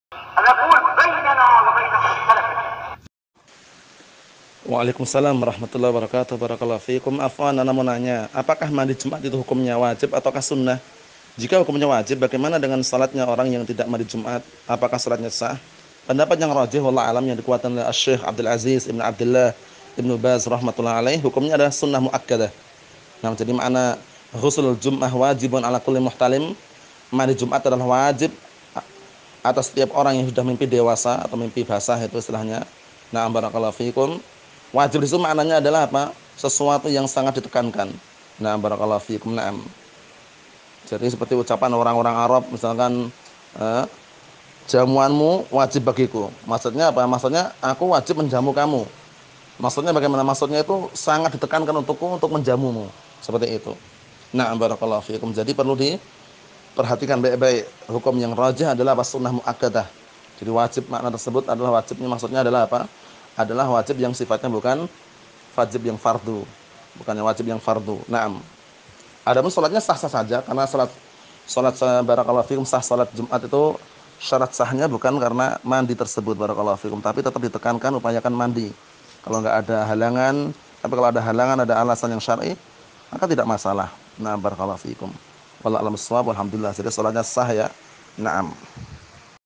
Apakah Hukum Mandi Jum'at Itu Wajib Atau Sunnah ?? | Ustadz Abu Zakaria Harits Al-Jabaly
▪︎Sesi Tanya Jawab bersama : Al~Ustadz Abu Zakaria Harits Al-Jabaly Hafidzahullah

Bismillah,..
Assalamu'alaikum warahmatullah...
📚 [ APAKAH HUKUM MANDI JUM'AT ITU WAJIB ATAU SUNNAH ??? ]
.Semoga bermanfaat 👍👍👍

USTADZABUZAKARIAHARITSAL-JABALY